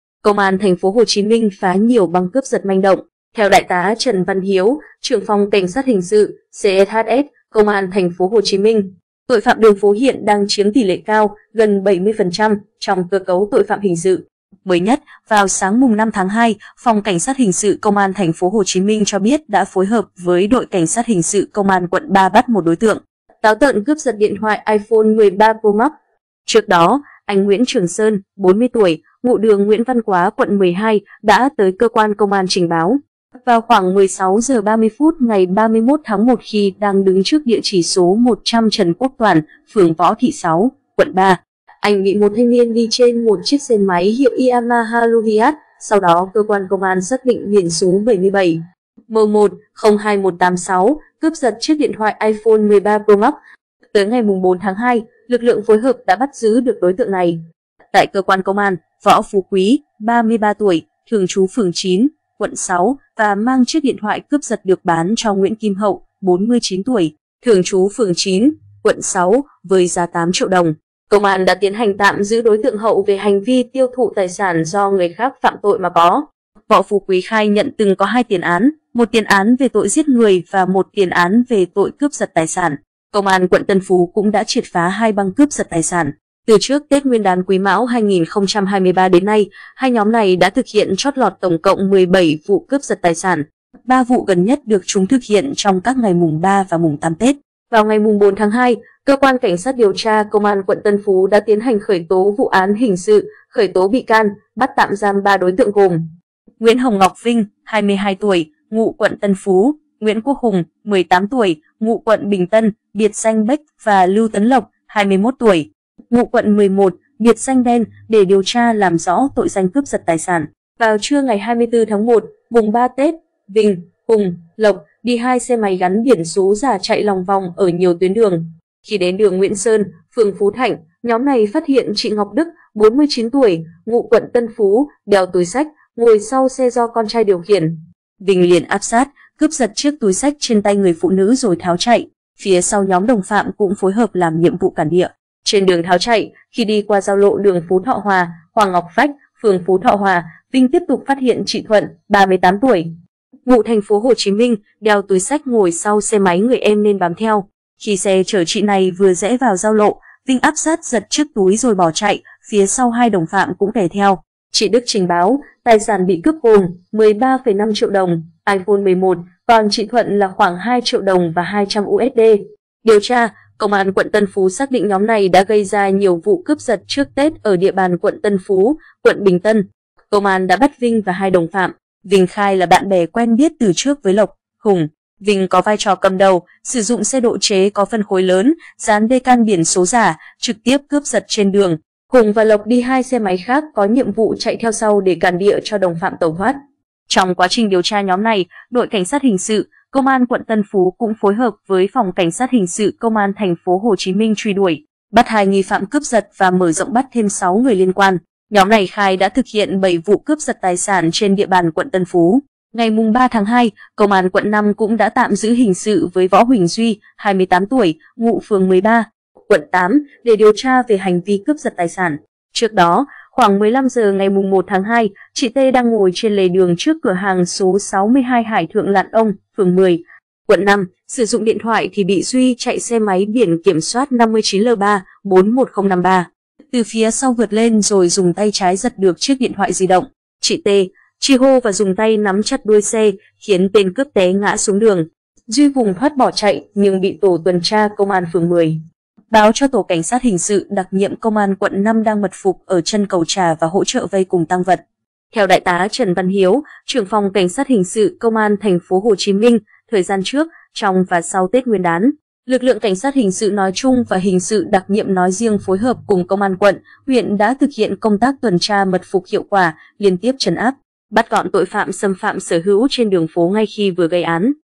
Công an TP Hồ Chí Minh phá nhiều băng cướp giật manh động
Theo Đại tá Trần Văn Hiếu, Trưởng phòng Cảnh sát hình sự (CSHS) Công an TP Hồ Chí Minh, tội phạm đường phố hiện đang chiếm tỷ lệ cao (gần 70%) trong cơ cấu tội phạm hình sự…Theo Đại tá Trần Văn Hiếu, Trưởng phòng CSHS Công an TP Hồ Chí Minh, thời gian trước, trong và sau Tết Nguyên đán, lực lượng CSHS nói chung và Hình sự đặc nhiệm nói riêng phối hợp cùng Công an quận, huyện đã thực hiện công tác tuần tra mật phục hiệu quả, liên tiếp trấn áp, bắt gọn tội phạm xâm phạm sở hữu trên đường phố ngay khi vừa gây án.
Võ Phú Quý khai nhận từng có hai tiền án - một tiền án về tội Giết người và một tiền án về tội Cướp giật tài sản.
Chị Đức trình báo, tài sản bị cướp gồm: 13,5 triệu đồng, iPhone 11, còn chị Thuận là khoảng 2 triệu đồng và 200 USD. Điều tra, Công an quận Tân Phú xác định nhóm này đã gây ra nhiều vụ cướp giật trước Tết ở địa bàn quận Tân Phú, quận Bình Tân... Công an đã bắt Vinh và hai đồng phạm. Vinh khai là bạn bè quen biết từ trước với Lộc, Hùng. Vinh có vai trò cầm đầu, sử dụng xe độ chế có phân khối lớn, dán decal biển số giả, trực tiếp cướp giật trên đường. Hùng và Lộc đi hai xe máy khác có nhiệm vụ chạy theo sau để "cản địa" cho đồng phạm tẩu thoát.
Trên đường tháo chạy, khi đi qua giao lộ đường Phú Thọ Hòa - Hoàng Ngọc Phách (phường Phú Thọ Hòa), Vinh tiếp tục phát hiện chị Thuận (38 tuổi, ngụ TP Hồ Chí Minh) đeo túi xách ngồi sau xe máy người em nên bám theo. Khi xe chở chị này vừa rẽ vào giao lộ, Vinh áp sát giật chiếc túi rồi bỏ chạy, phía sau hai đồng phạm cũng kè theo.
Trong quá trình điều tra nhóm này, Đội CSHS, Công an quận Tân Phú cũng phối hợp với Phòng CSHS Công an TP Hồ Chí Minh truy đuổi, bắt hai nghi phạm cướp giật và mở rộng bắt thêm 6 người liên quan. Nhóm này khai đã thực hiện 7 vụ cướp giật tài sản trên địa bàn quận Tân Phú.
Vào trưa 24/1 (mùng 3 Tết), Vinh, Hùng, Lộc, đi hai xe máy gắn biển số giả chạy lòng vòng ở nhiều tuyến đường. Khi đến đường Nguyễn Sơn, phường Phú Thạnh, nhóm này phát hiện chị Ngọc Đức (49 tuổi, ngụ quận Tân Phú) đeo túi xách, ngồi sau xe do con trai điều khiển. Vinh liền áp sát, cướp giật chiếc túi xách trên tay người phụ nữ rồi tháo chạy. Phía sau nhóm đồng phạm cũng phối hợp làm nhiệm vụ cản địa.
Mới nhất, vào sáng 5/2, Phòng CSHS Công an TP Hồ Chí Minh cho biết đã phối hợp với Đội CSHS Công an quận 3 bắt một đối tượng táo tợn cướp giật điện thoại Iphone 13 Promax…
Ngày 3/2, Công an quận 5 cũng đã tạm giữ hình sự với Võ Huỳnh Duy (28 tuổi, ngụ phường 13, quận 8) để điều tra về hành vi cướp giật tài sản. Trước đó, khoảng 15h ngày 1/2, chị T. đang ngồi trên lề đường trước cửa hàng số 62 Hải Thượng Lãn Ông (phường 10, quận 5) sử dụng điện thoại thì bị Duy chạy xe máy BKS 59L3-410.53 từ phía sau vượt lên rồi dùng tay trái giật được chiếc điện thoại di động. Chị T. tri hô và dùng tay nắm chặt đuôi xe, khiến tên cướp té ngã xuống đường. Duy vùng thoát bỏ chạy nhưng bị tổ tuần tra Công an phường 10, báo cho Tổ CSHS đặc nhiệm Công an quận 5 đang mật phục ở chân cầu Chà Và hỗ trợ vây cùng tang vật...
Trước đó, anh Nguyễn Trường Sơn (40 tuổi; ngụ đường Nguyễn Văn Quá quận 12) đã tới cơ quan Công an trình báo, vào khoảng 16h30 ngày 31/1 khi đang đứng trước địa chỉ số 100 Trần Quốc Toản, phường Võ Thị Sáu, quận 3, anh bị một thanh niên đi trên một chiếc xe máy hiệu Yamaha Luvias (sau đó cơ quan Công an xác định biển số 77M1-021.86) cướp giật chiếc điện thoại Iphone 13 Promax.
Vào ngày 4/2, Cơ quan CSĐT Công an quận Tân Phú đã tiến hành khởi tố vụ án hình sự, khởi tố bị can, bắt tạm giam 3 đối tượng gồm: Nguyễn Hồng Ngọc Vinh (22 tuổi, ngụ quận Tân Phú), Nguyễn Quốc Hùng (18 tuổi, ngụ quận Bình Tân, biệt danh Bake) và Lưu Tấn Lộc (21 tuổi, ngụ quận 11, biệt danh Đen) để điều tra làm rõ tội danh Cướp giật tài sản.
Các đối tượng cướp giật bị bắt cùng tang vật.
Tới ngày 4/2, lực lượng phối hợp đã bắt giữ được đối tượng này. Tại cơ quan Công an, Võ Phú Quý (33 tuổi, thường trú phường 9, quận 6) và mang chiếc điện thoại cướp giật được bán cho Nguyễn Kim Hậu (49 tuổi, thường trú phường 9, quận 6) với giá 8 triệu đồng. Công an đã tiến hành tạm giữ đối tượng Hậu về hành vi tiêu thụ tài sản do người khác phạm tội mà có.
Công an quận Tân Phú cũng đã triệt phá hai băng cướp giật tài sản. Từ trước Tết Nguyên đán Quý Mão 2023 đến nay, 2 nhóm này đã thực hiện trót lọt tổng cộng 17 vụ cướp giật tài sản. Ba vụ gần nhất được chúng thực hiện trong các ngày mùng 3 và mùng 8 Tết.